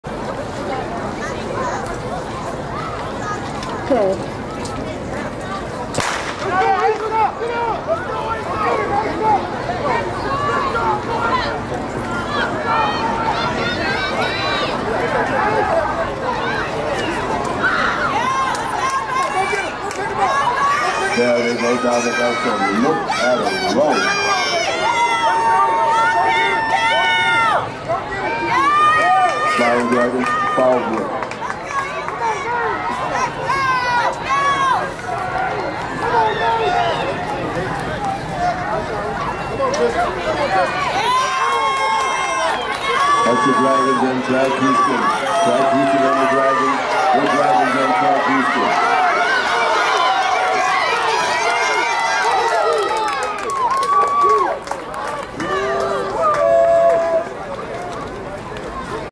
Track Houston 2023 Championship Meet: 13-14 Division Boys 4X100 Meter Final H2
2023 TRACK HOUSTON CHAMPIONSHIP MEET
June 3-4, 2023
Turner Stadium
Humble, Texas